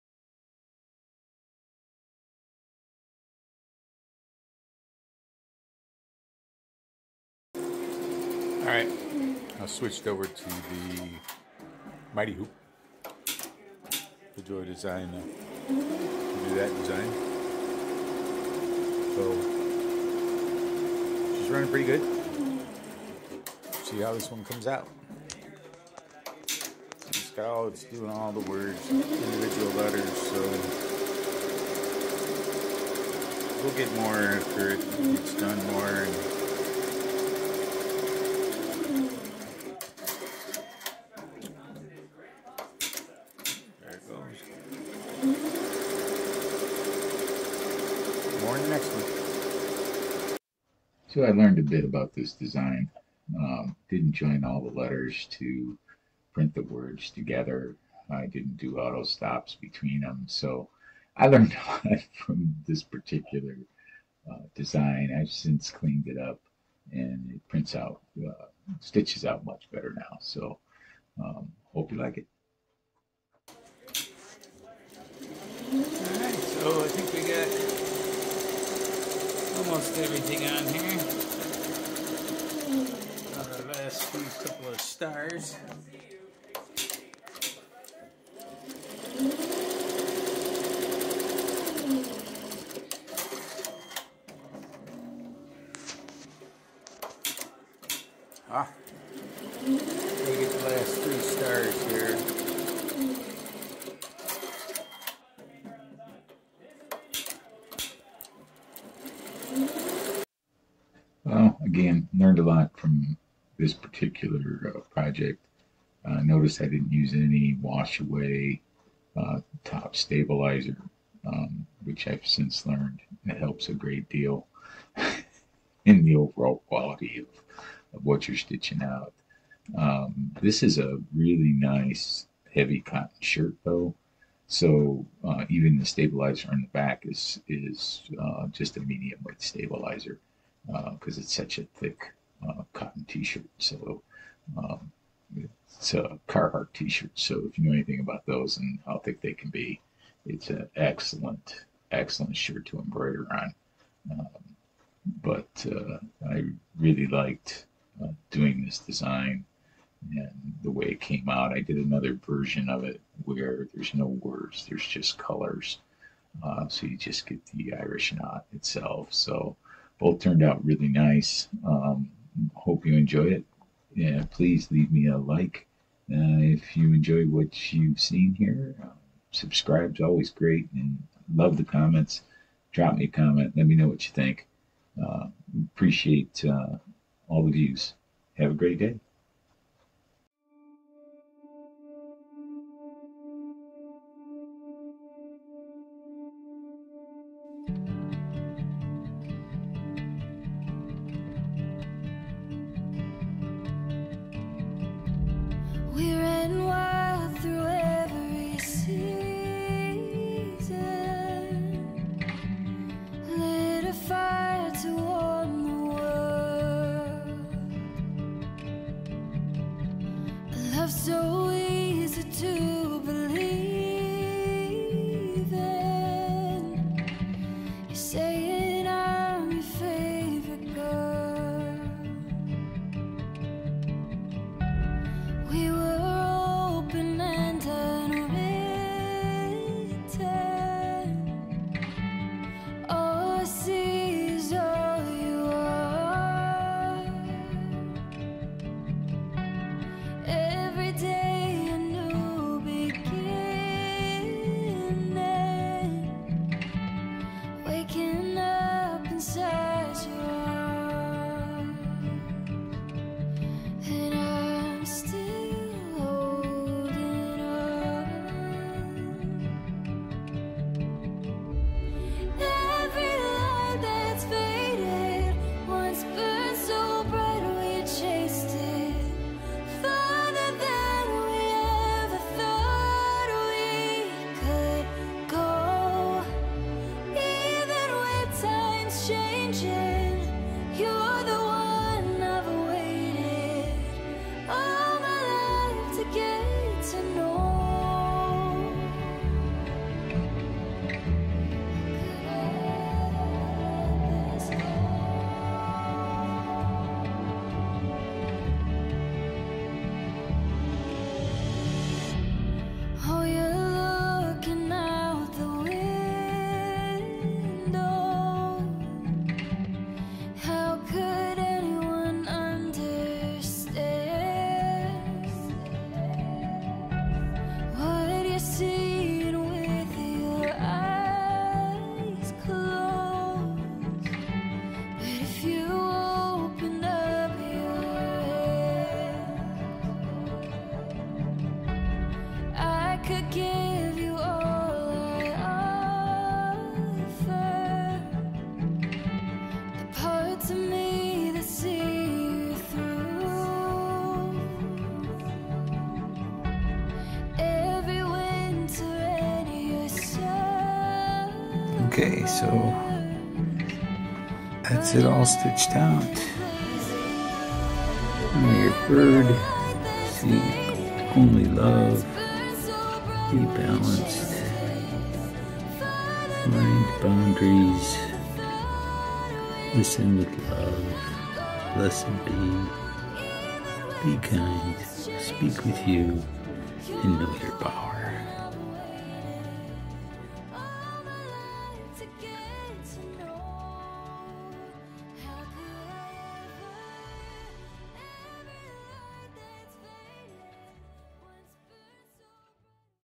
Irish Knot with Words
An Irish Knot with words that my wife liked so I made this design for her.